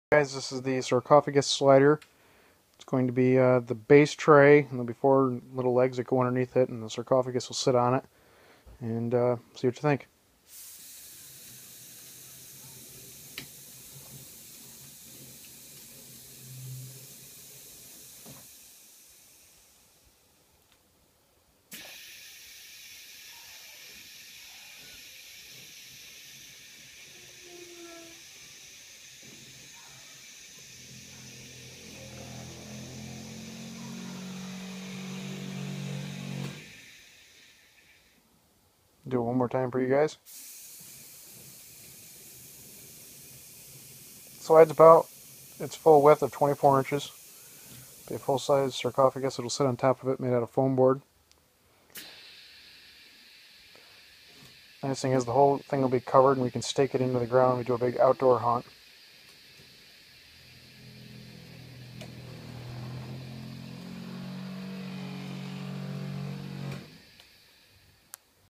sarcophagus sliding halloween prop - operation
This is part of a series of videos that shows the operation of our sarcophagus sliding prop for our scare. Later I will upload another video with paint and stands installed prior to putting the sarcophagus on.